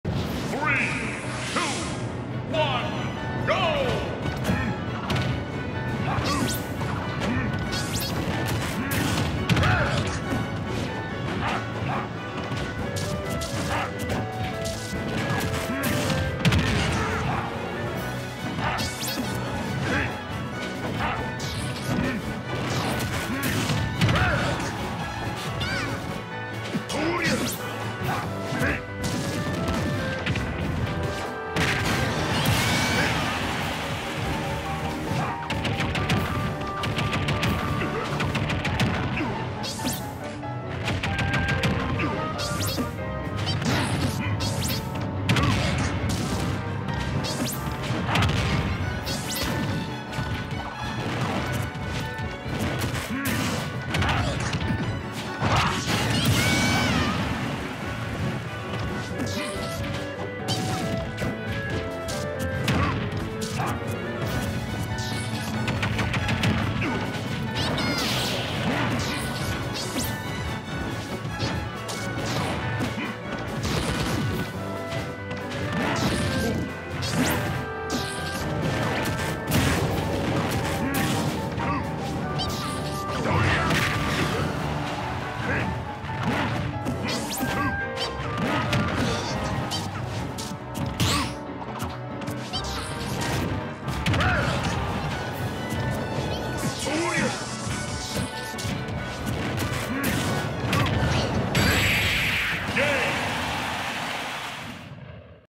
Try and take at least 1 stock
Try and take atleast 1 stock

I should have lost at least 1 stock when I was offstage but my recoveries were on point. Always practice your recovery game with Ganondorf, you'll need it.